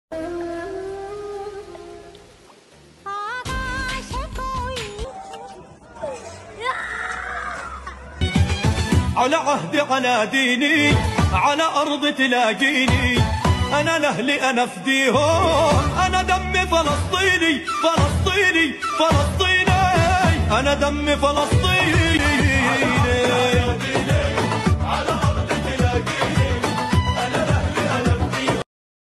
Şanlıurfalı sosyal medya fenomeni Pınar Cılıt'ın cesur hareketi!
Videoda yüzünü müziğin ritmine göre Filistin bayrağına boyayan Cılıt'in yaptığı akım videosu kısa sürede binlerce beğeni aldı ve halk tarafından takdir edildi.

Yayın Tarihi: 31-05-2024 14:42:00